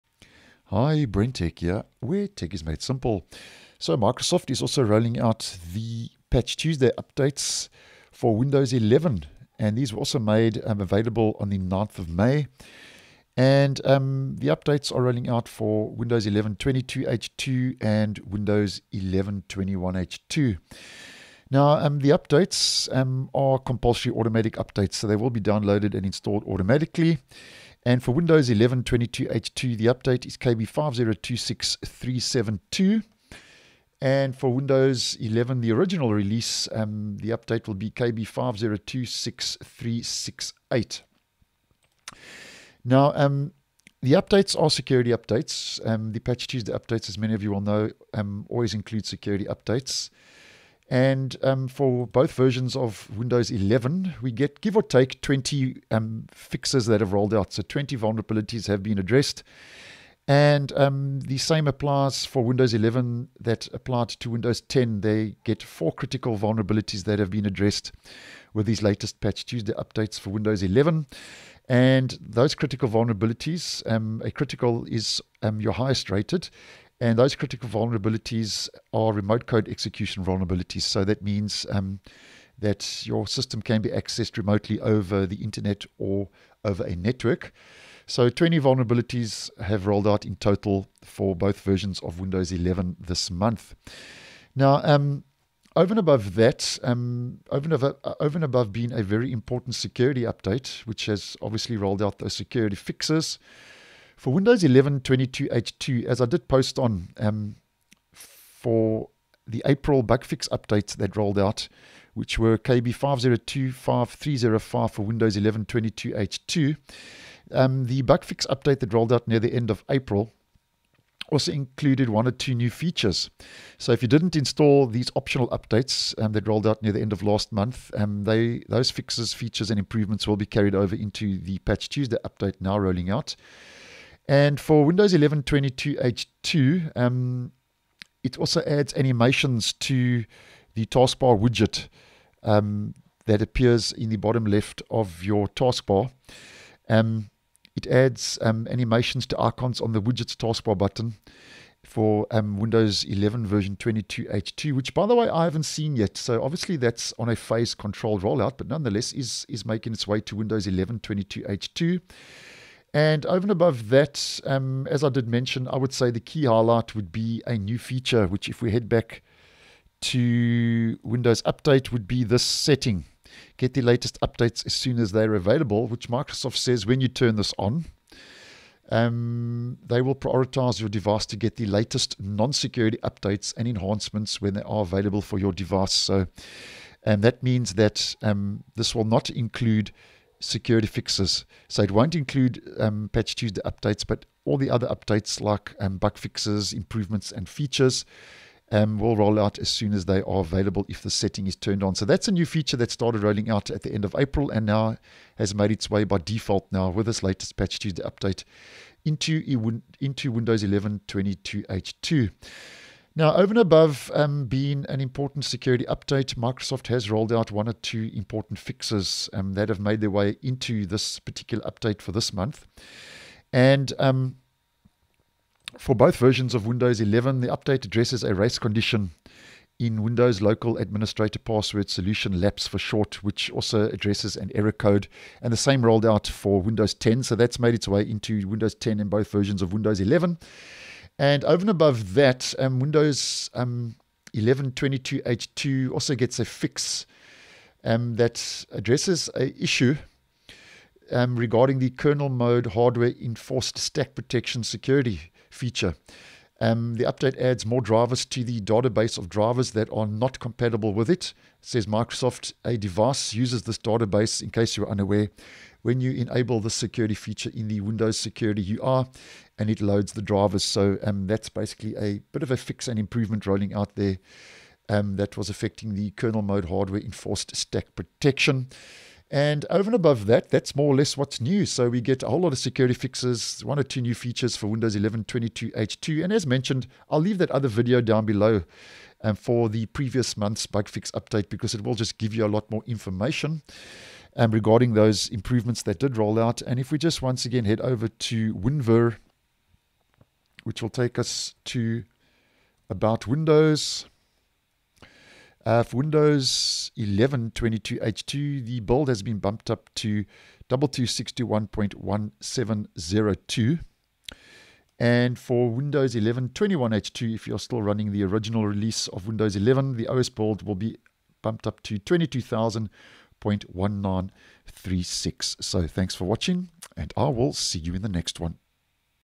Windows 11 22H2 KB5026372 and 21H2 KB5026368 released with New features, Security fixes & more
These latest May "B" release Patch Tuesday security updates for Windows 11 address 20 vulnerabilities. The update also includes a new toggle control on the Windows Update page to receive future non-security updates as soon as they are available, adds animations to icons on the Widgets taskbar button and more in version 22H2. Both versions also receive improvements and numerous bug fixes The update pushes Windows 11 22H2 to Build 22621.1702 and Windows 11 21H2 to Build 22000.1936.